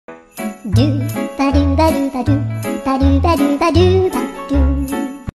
Cute Face Of Puffer Fish | Smiling Fish
Puffer fish may contain the potent and deadly toxins tetrodotoxin and/or saxitoxin which can cause severe illness and death. These are central nervous system toxins and are more deadly than cyanide. Symptoms start within 20 minutes to 2 hours after eating the toxic fish.